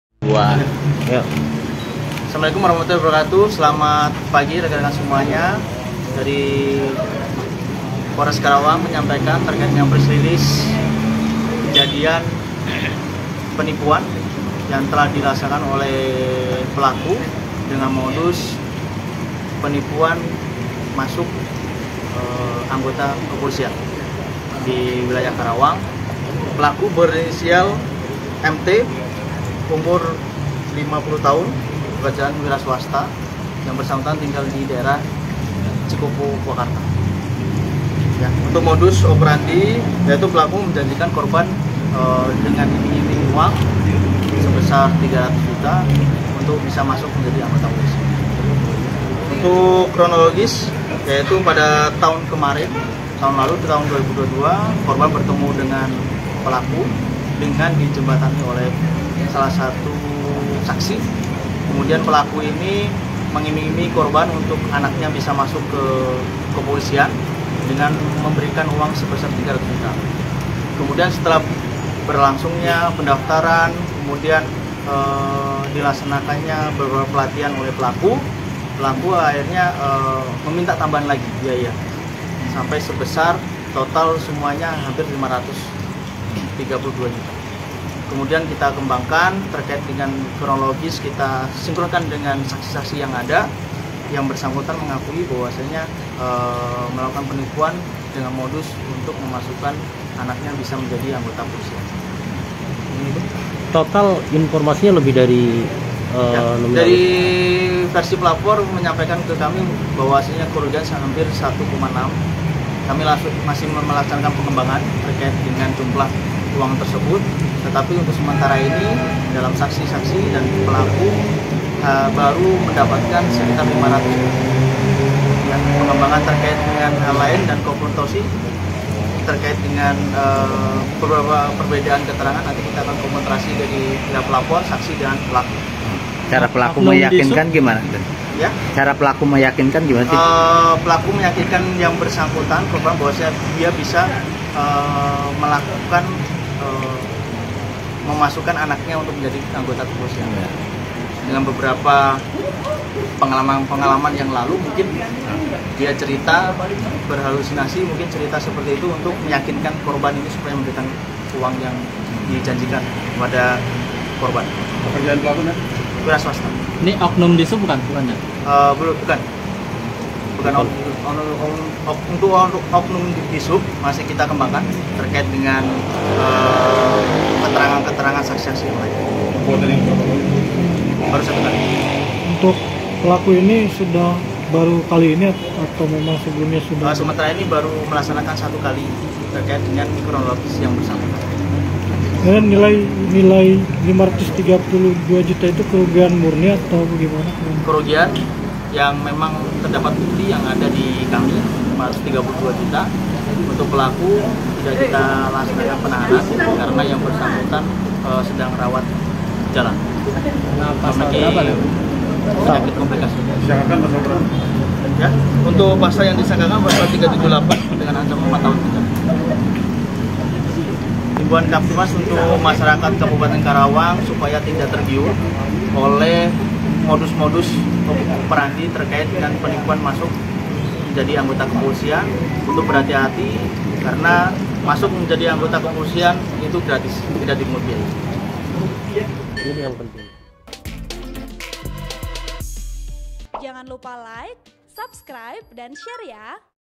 DITIPU RP 500 JUTA ANAK BISA MASUK POLRI, EH TERNYATA ZONK!
Laporan Kontributor Tribunjabar.id, Karawang, Cikwan Suwandi
|Video Editor: Uben